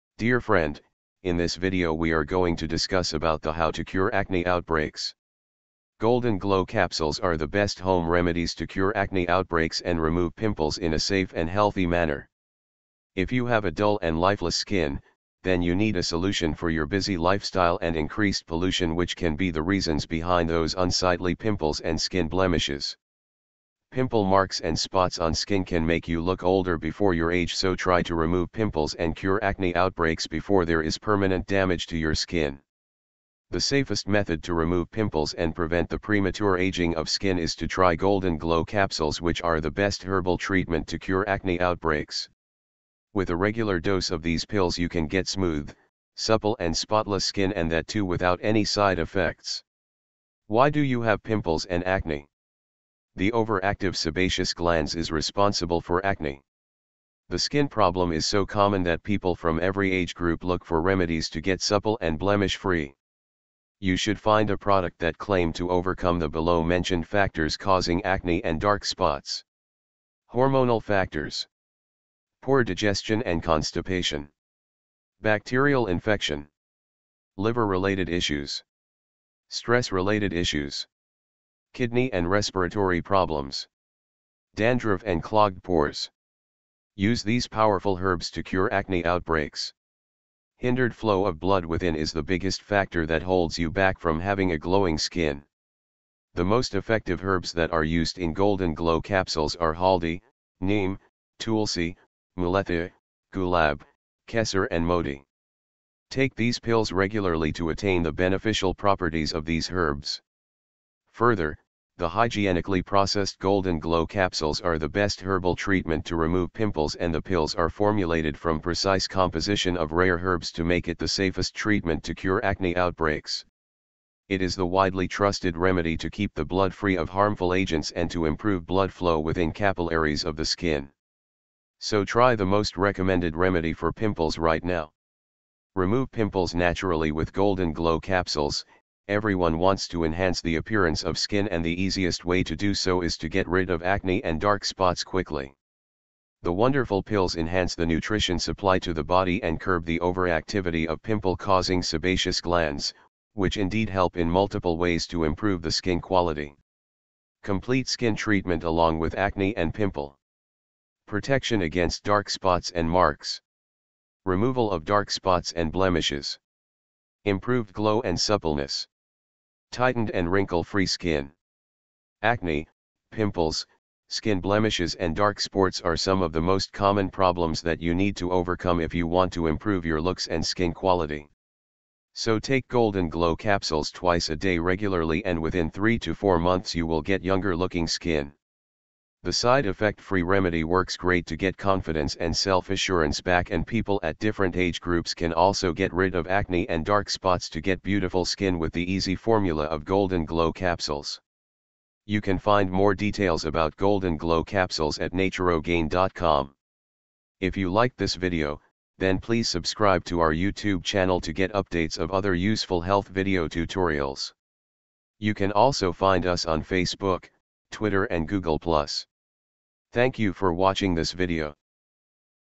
How To Cure Acne Outbreaks And Remove Pimples With Home Remedies?
Dear friend, in this video we are going to discuss about the how to cure acne outbreaks. Golden Glow capsules are the best home remedies to cure acne outbreaks and remove pimples in a safe and healthy manner.

How To Cure Acne Outbreaks